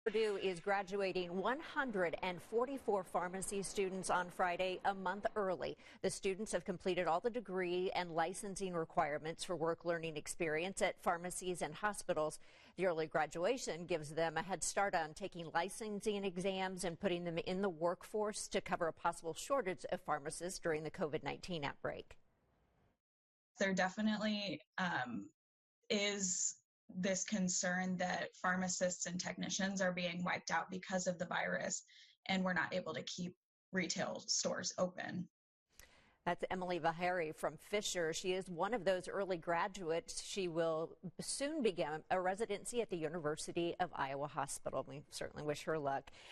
Purdue pharmacy students graduate early
Hundreds of Purdue pharmacy students are graduating early as concerns about pharmacists and pharmacy techs being infected with coronavirus spread.